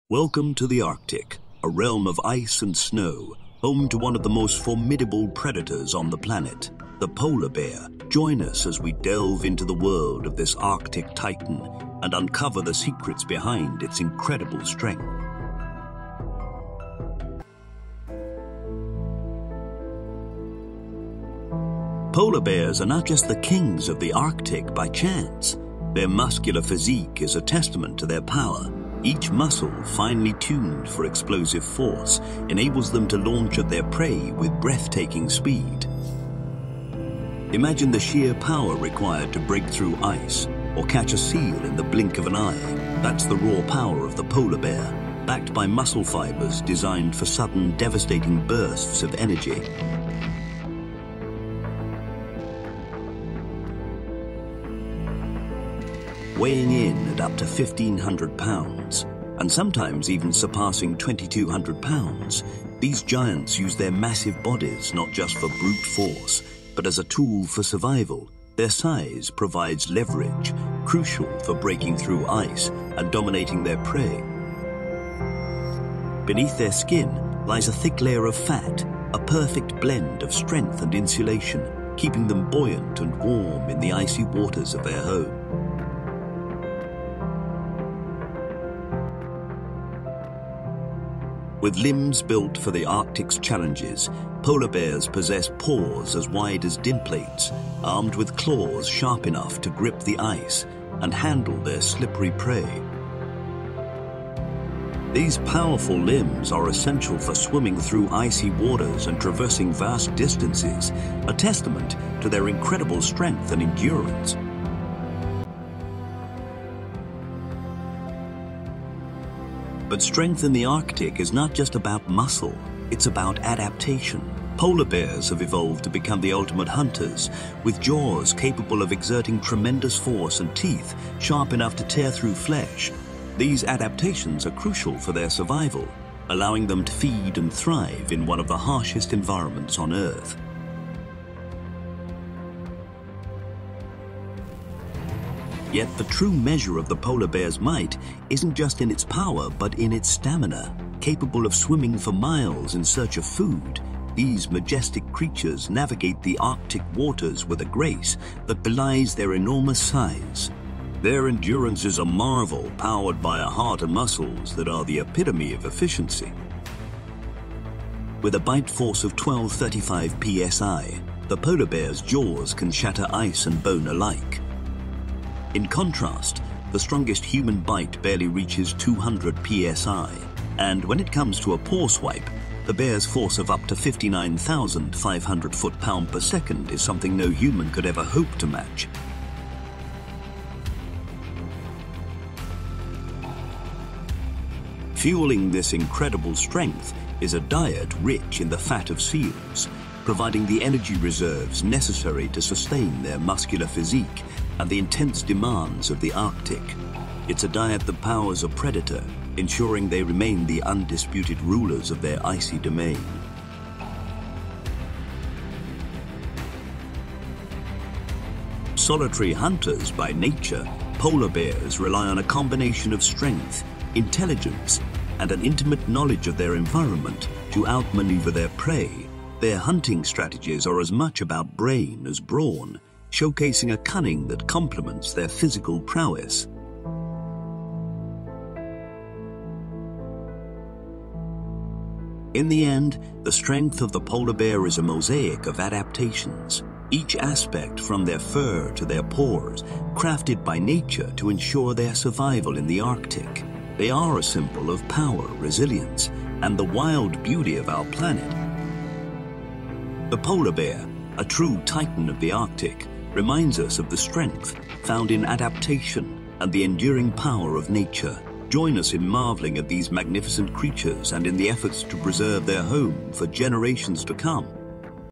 How Strong is a Polar Bear? | Size, Weight, Bit Force and Adaptation.
How strong is a polar bear? This video shows the physical and biological features that make polar bears the largest living carnivores on Earth. You will learn about:

•  Muscle mass and composition: How polar bears hunt, swim, and fight with their powerful muscles.

•  Body size, weight and structure: How big are polar bears and how their body shape helps them survive in the Arctic.

•  Limbs and joint configuration: How polar bears move on land and in water with their adapted limbs and joints.

•  Adaptations: How polar bears cope with the cold, low light, and scarce food of their habitat.

•  Endurance and stamina: How long polar bears can go without eating and how they conserve energy.

•  Bite force of the polar bear: How strong is a polar bear's bite and what it can do with its teeth and jaws.

•  Diet and nutrition: What polar bears eat and how they get nutrients from their prey.

•  Behavioural factors: How polar bears communicate, socialize, and compete with each other.

•  Environmental adaptation: How climate change affects polar bears and what are the threats and challenges they face.

Watch this video to discover why polar bears are the kings of the Arctic.

Chapters:
00:00 - Introduction
00:21 - Muscle Mass and Composition
00:52 - Body Size, Weight and Structure
01:31 - Limbs and Joint Configuration
01:57 - Adaptations
02:23 - Endurance and Stamina
02:48 - Bite force of the Polar bear
03:14 - Diet and Nutrition03:36 - Behavioural Factors
03:58 - Environmental Adaptation